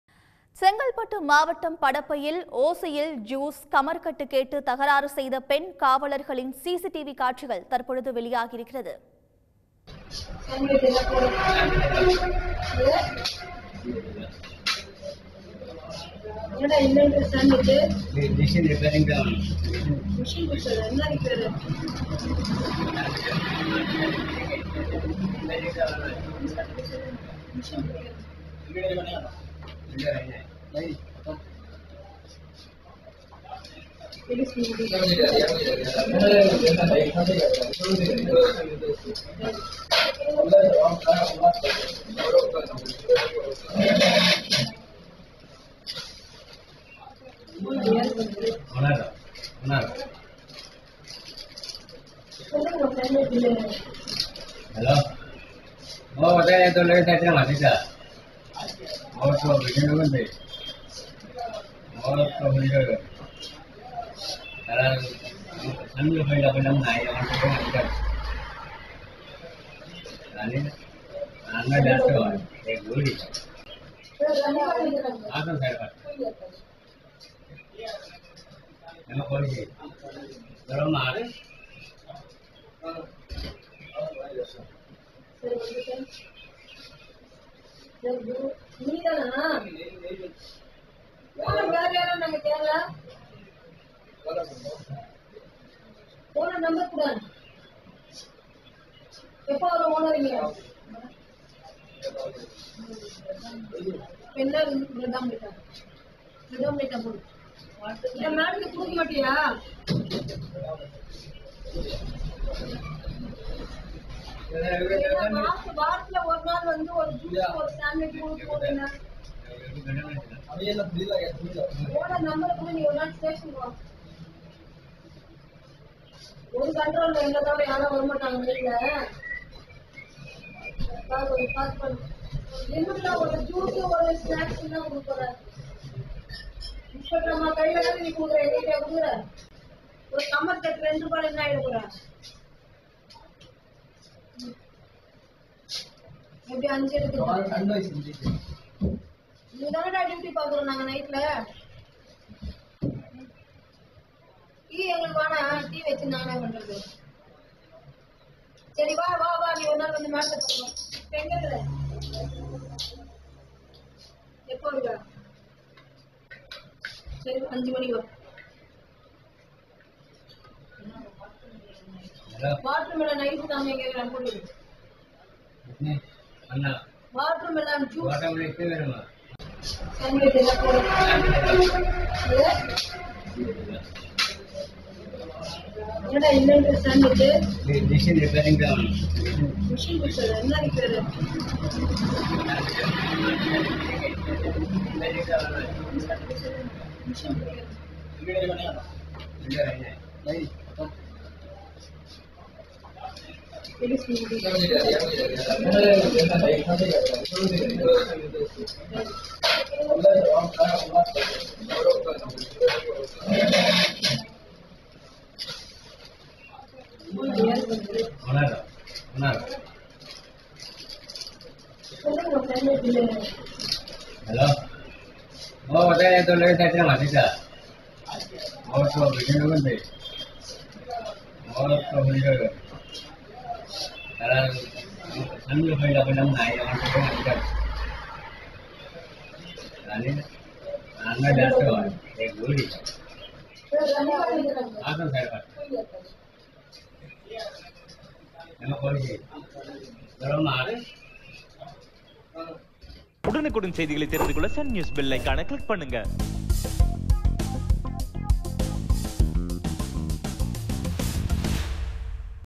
ஓசியில் Juice, Kamarkattu கேட்டு தகராறு செய்த 3 பெண் காவலர்கள் -வெளியான அதிர்ச்சி சிசிடிவி காட்சிகள்
ஓசியில் ஜூஸ்,கமர்கட் கேட்டு தகராறு செய்த 3 பெண் காவலர்கள் - வெளியான அதிர்ச்சி சிசிடிவி காட்சிகள் | Sun News